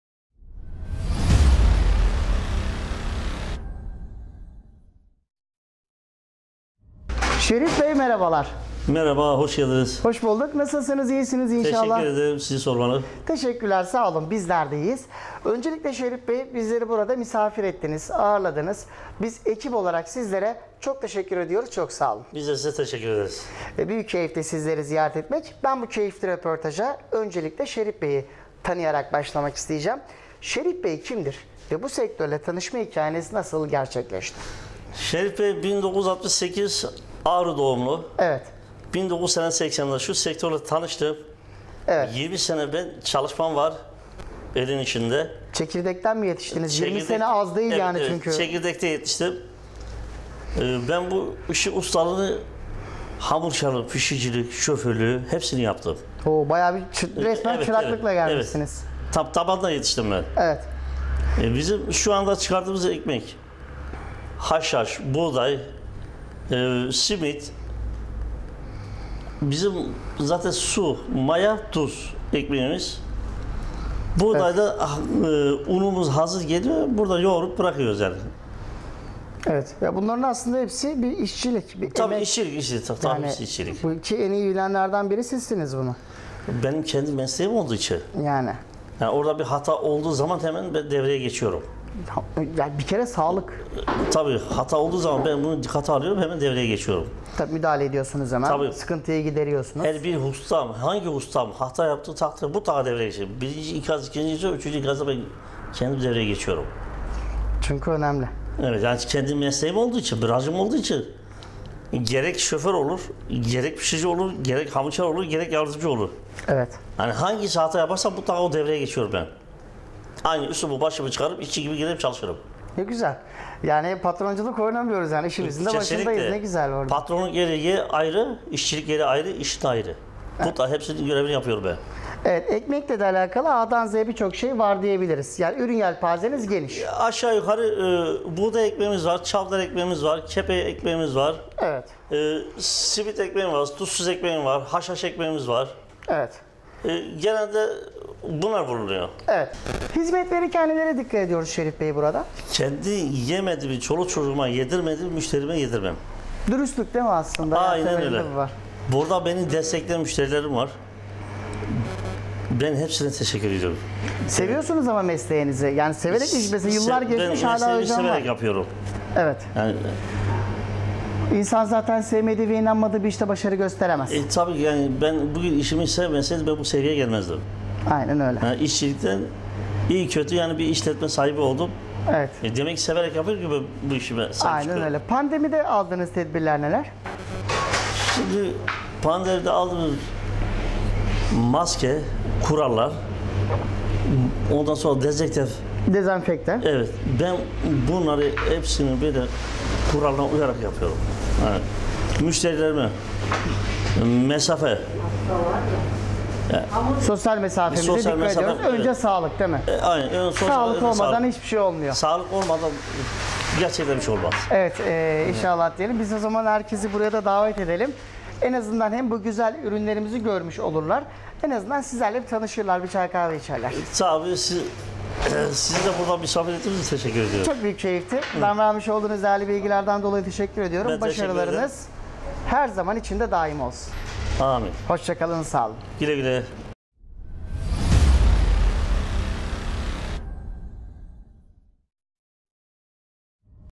BEYLİKBAĞI TADIM EKMEK FIRINI - GEBZE/KOCAELİ
BEYLİKBAĞI TADIM EKMEK FIRINI firma sahibi Şerif YILDIZ, başarı ve faaliyetlerini Konya Haber Tv muhabirimiz Sinan TOPÇUOĞLU' na anlattı.